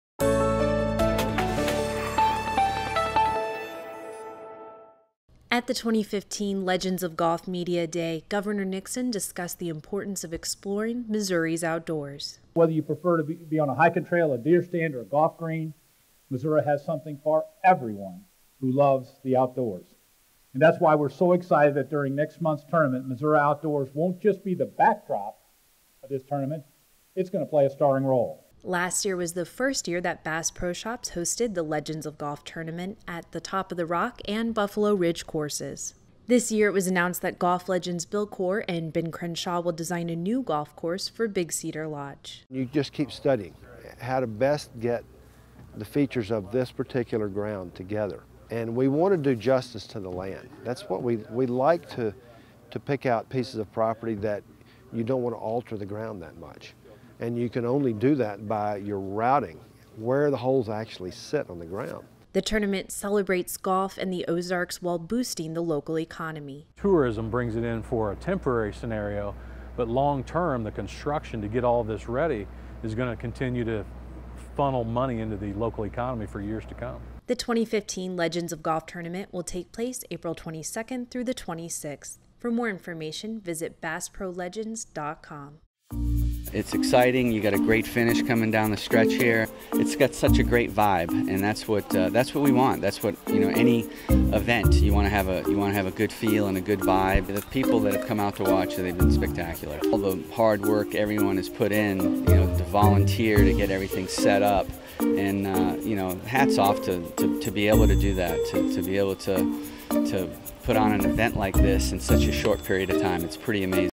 2015 Legends of Golf Media Day
Gov. Nixon joined Bass Pro Shops' Johnny Morris to discuss the 2015 Legends of Golf Tournament.